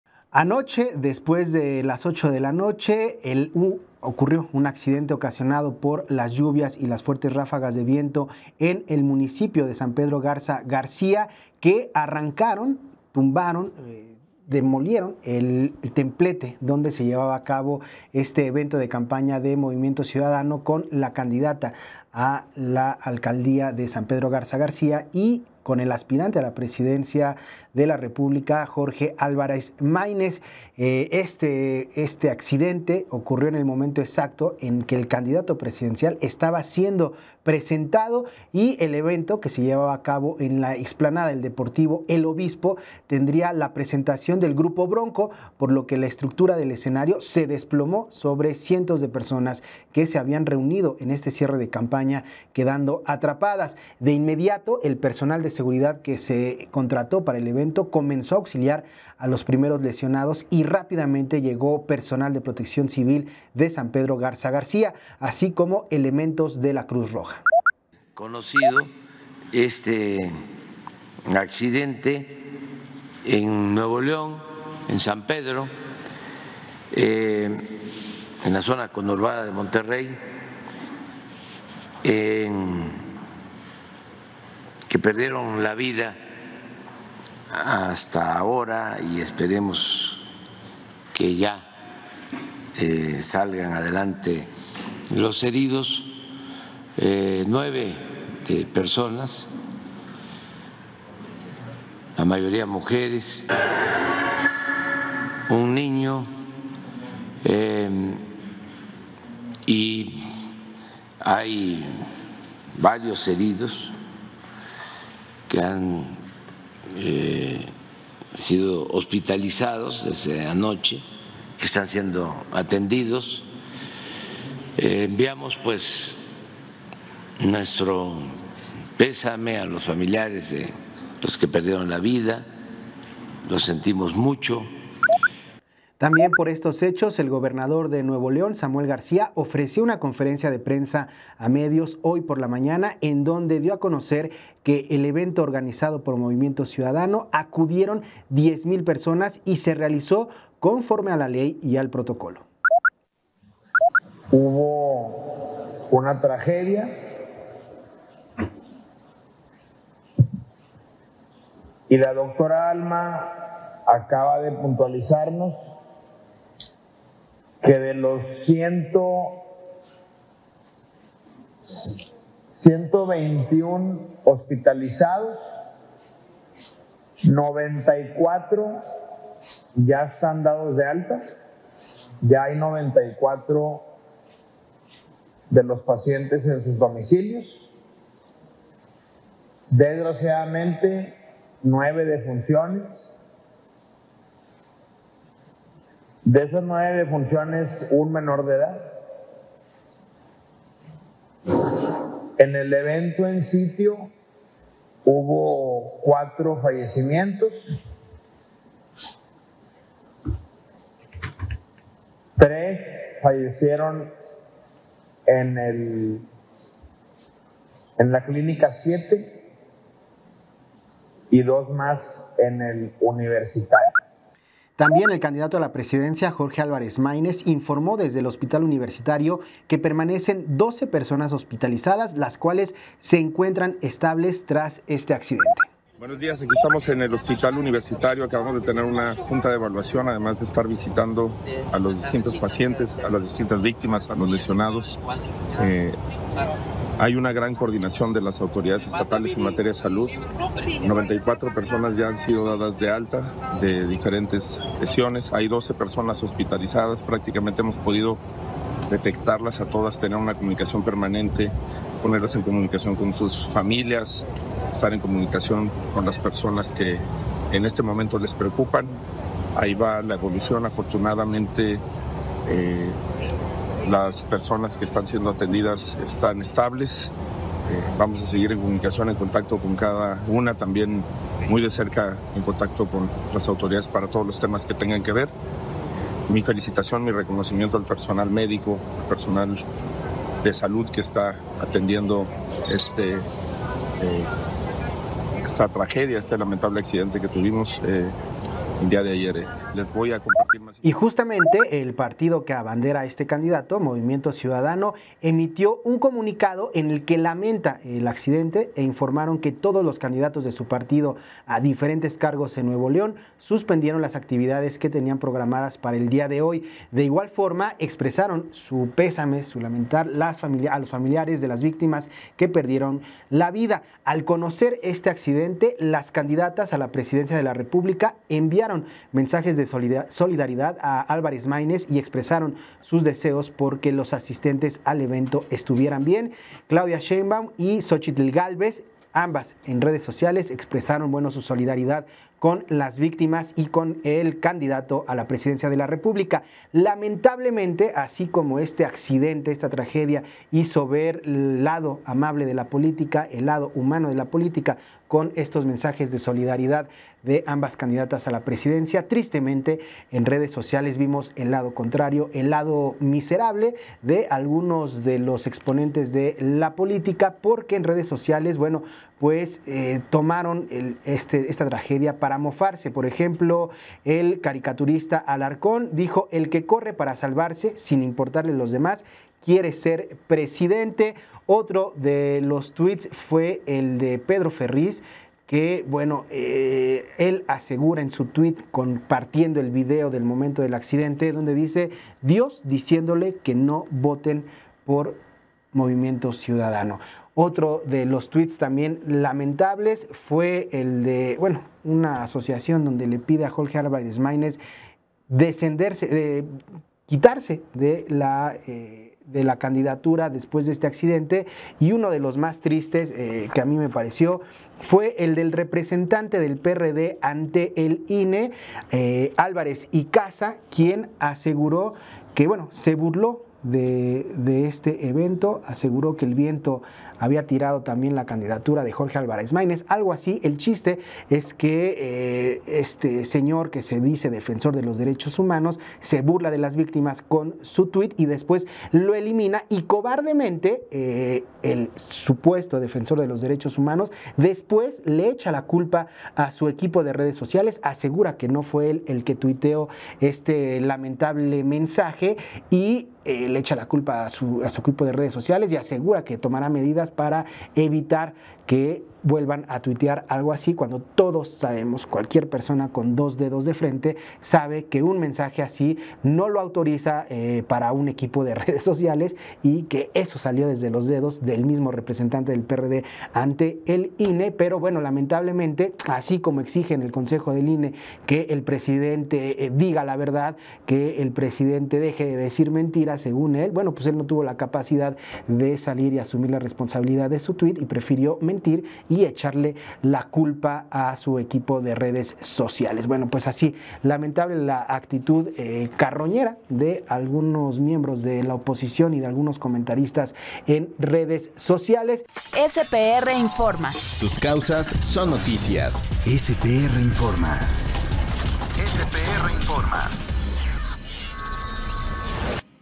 Crónica del derrumbe del templete de Movimiento Ciudadano en San Pedro Garza García
SPRInforma | Se derrumbó el templete del cierre de campaña de Lorenia Canavati, candidata de Movimiento Ciudadano a la alcaldía de San Pedro GarzaGarcia en NuevoLeón, y en el que también se encontraba el candidato presidencial Álvarez Máynez.

El gobernador de la entidad, Samuel García, informó que, al corte de las 7:00 horas de este 23 de mayo, había un total de 9 personas fallecidas, 121 lesionados y 27 hospitalizados.